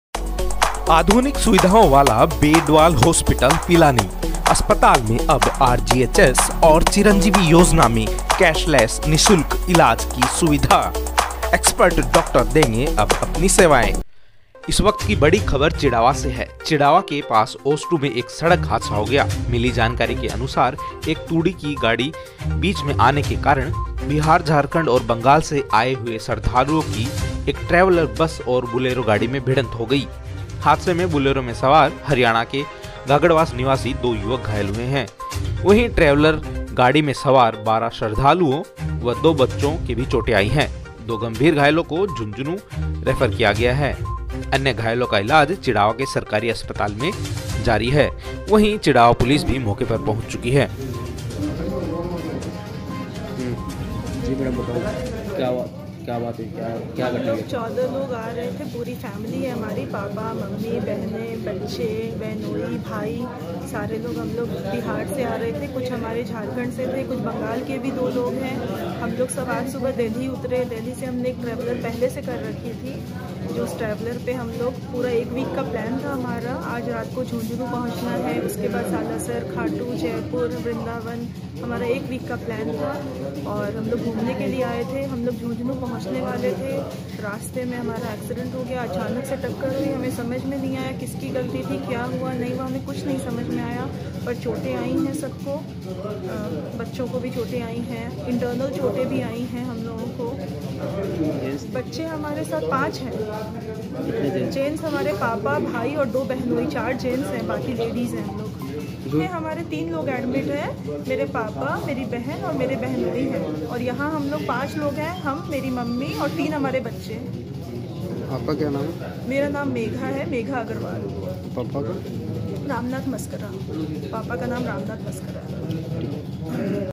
चिड़ावा में सड़क हादसा 10-12 लोग घायल होने की खबर,2 को झुंझुनू किया रेफ़र, बोलेरो और ट्रेवलर में टक्कर
चिड़ावा ब्रेकिंग न्यूज़ / ट्रेवलर और बुलेरो में भिड़ंत,कई घायल